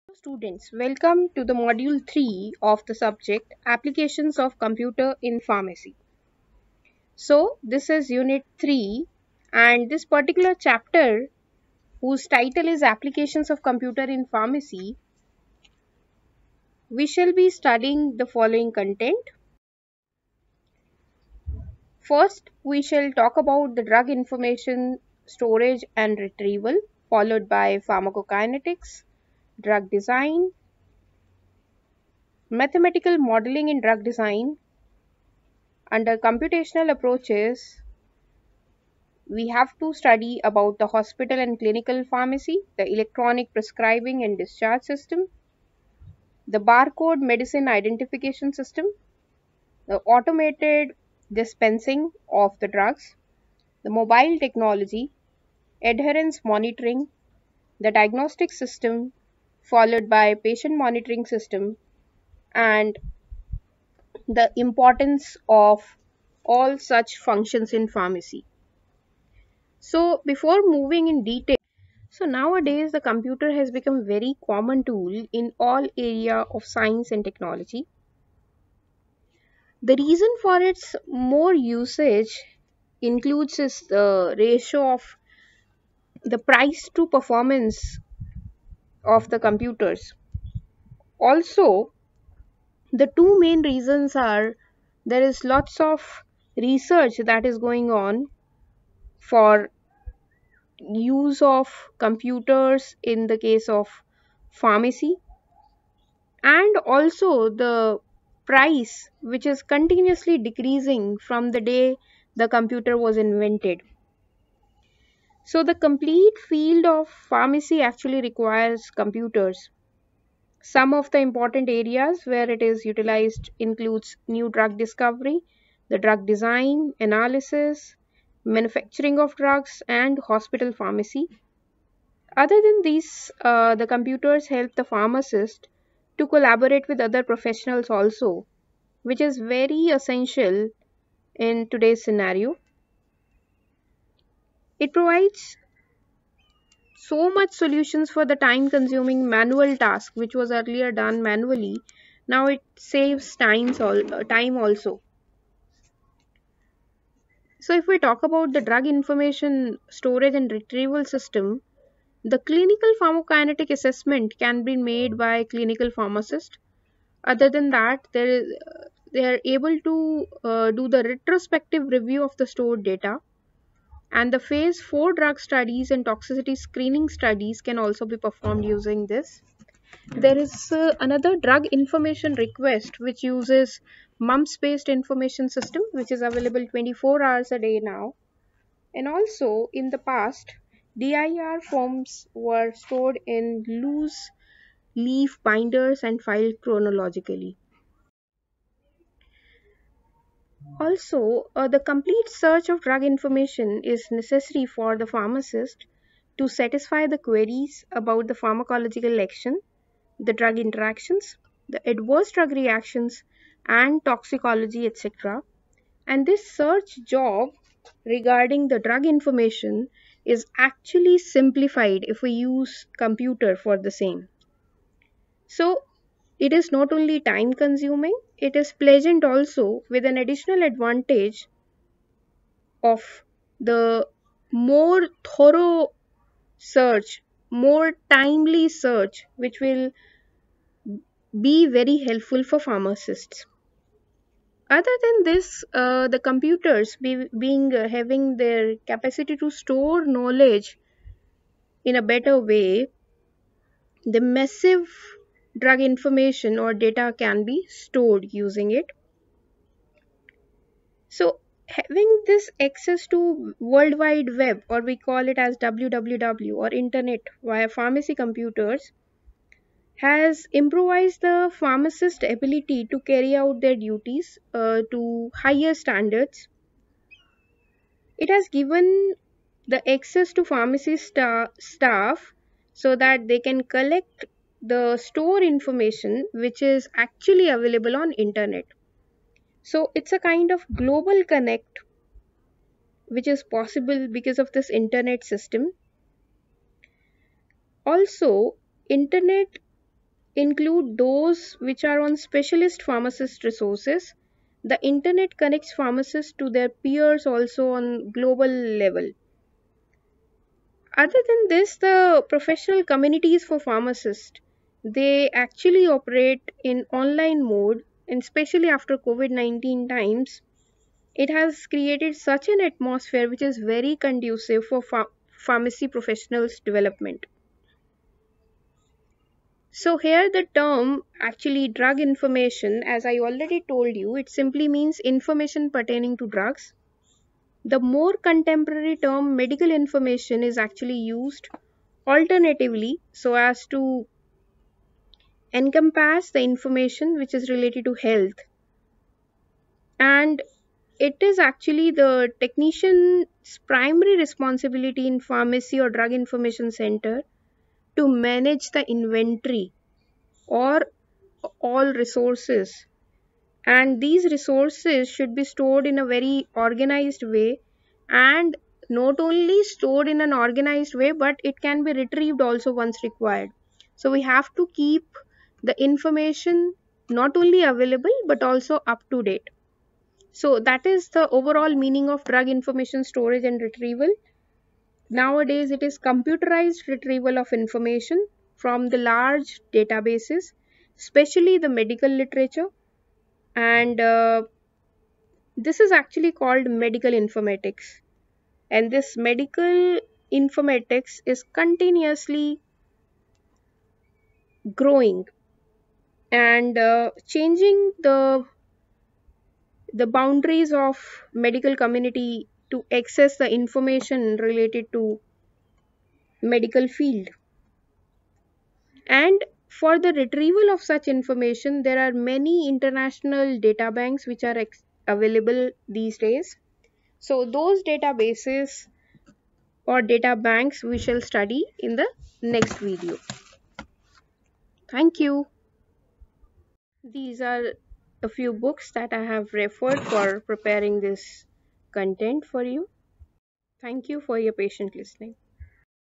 Module Lecture 2 Drug information storage and retrieval
Applications of computer in Drug information storage and retrieval is explained